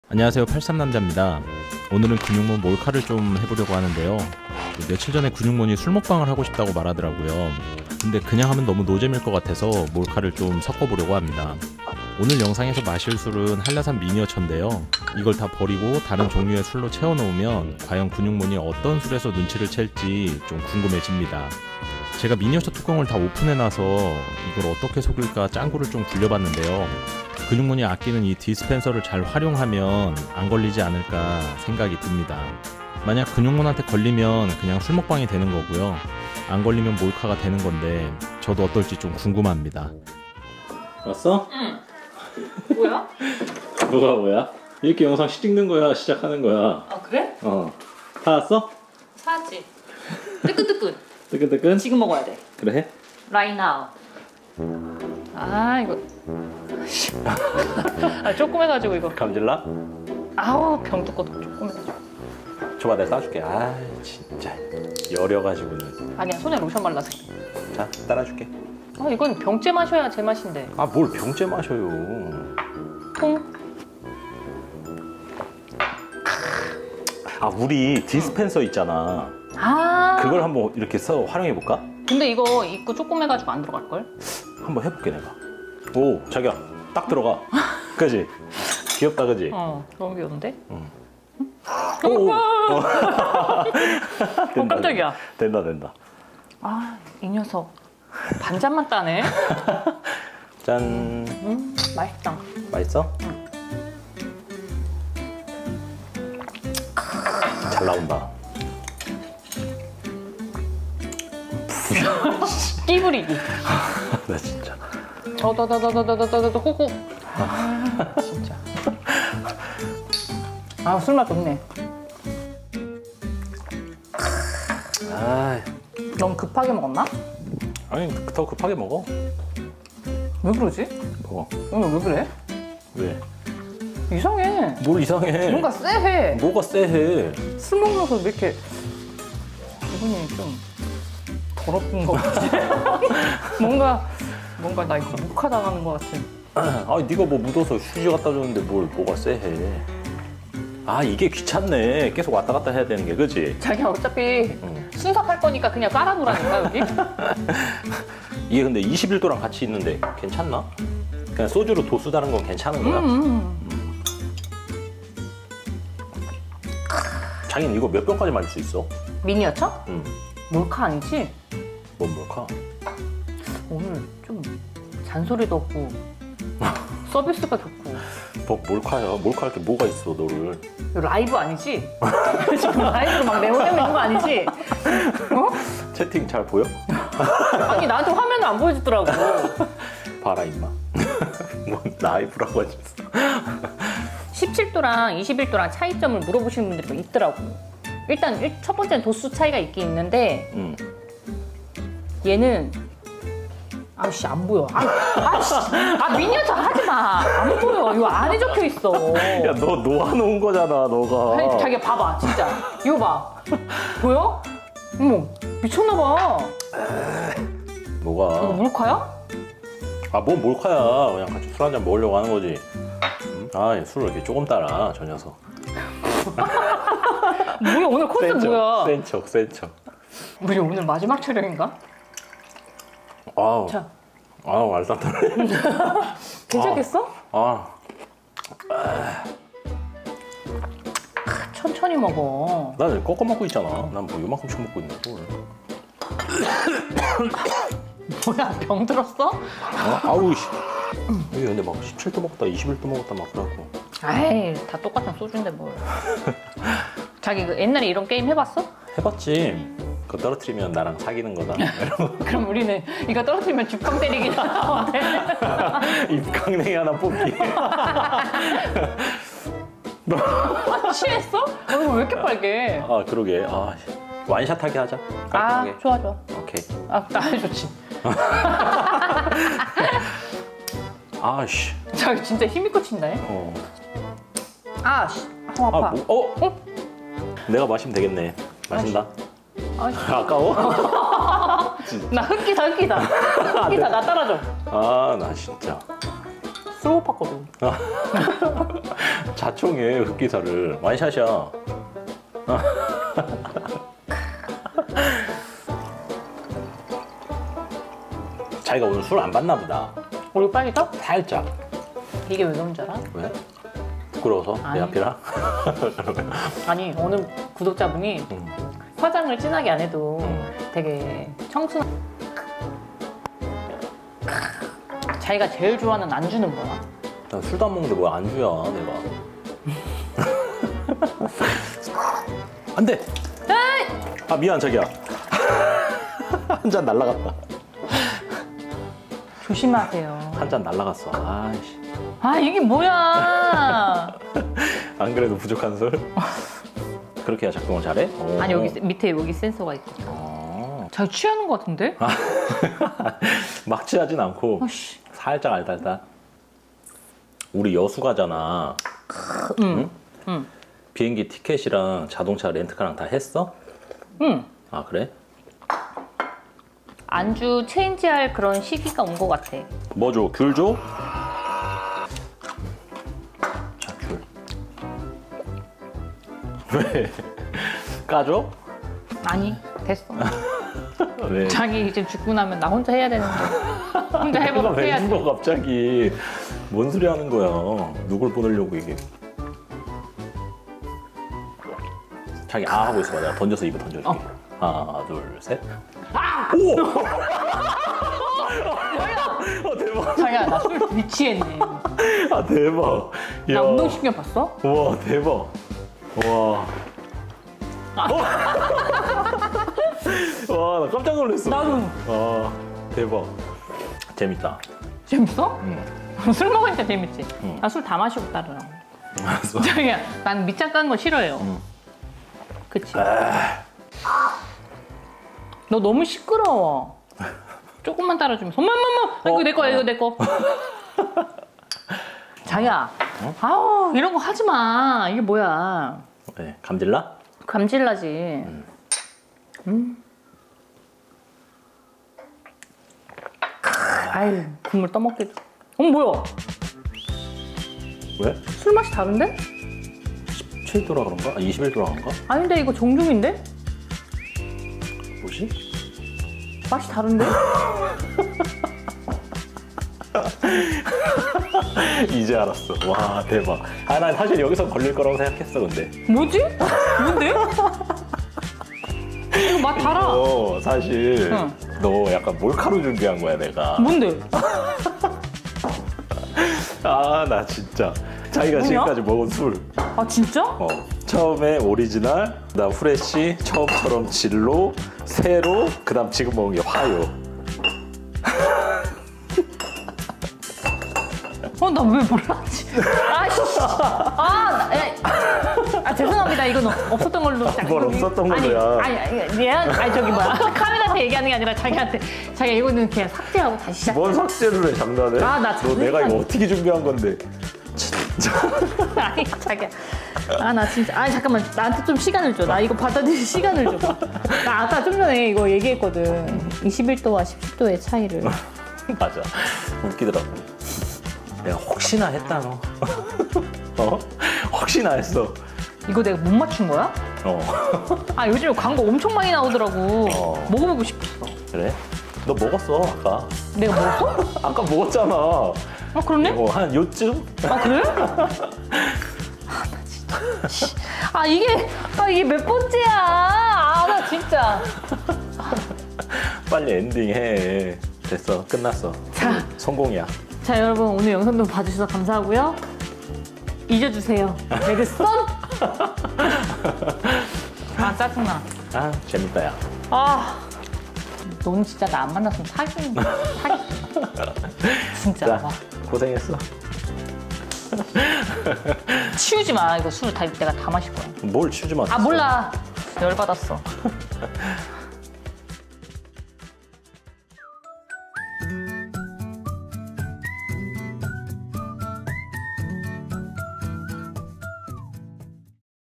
술 좋아하는 아내.. 바보 만들기ㅋㅋ
안녕하세요 주신님들😊
이번 영상은 음... 약간 짜증이 밀려오는 영상이네요 ㅎㅎ
저는 그냥 술먹방을 하자고 했는데
저 남자는 몰카를 준비 했더라구요 ㅠㅠ
다른 술은 몰라도 소주는 제가 한 자부심 하는데... 하...
이제는 술 맛을 모르는 걸로 해야할 것 같아요 ㅎㅎ
술은 그냥 맛보단 취하기 위해 먹는 걸로~~~ ^^
술 먹방에서 몰카로 변하긴 했지만 그래도 영상으로 보니 웃기네요 ^^;;
우리 주신님들 이번 영상도 재밌게 봐주셨음 좋겠습니다
주신님들 이번 한주도 고생 많으셨구요
저희는 그럼 다음주에 다시 만나요^^